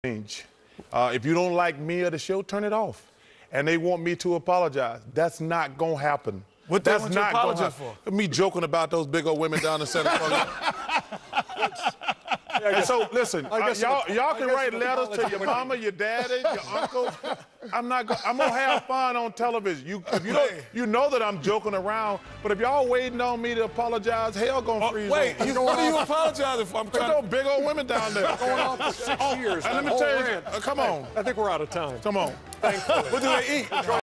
Charles Barkley refuses to apologize for comments about San Antonio's women
TNT commentator Charles Barkley refuses to apologize for comments made about the weight of women in San Antonio, Texas.